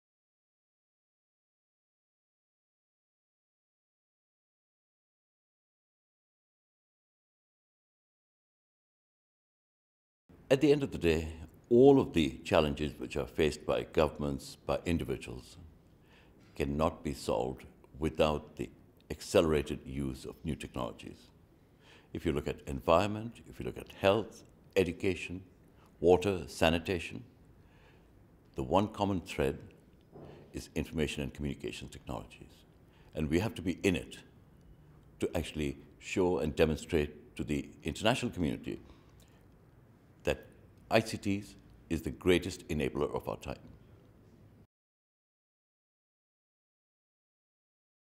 ITU INTERVIEWS: Amir Dossal, Chairman, Global Partnerships Forum
Interview with Amir Dossal, Chairman of the Global Partnerships Forum, at the Broadband Commission for Digital Development Meeting, Dublin, 23 March 2014.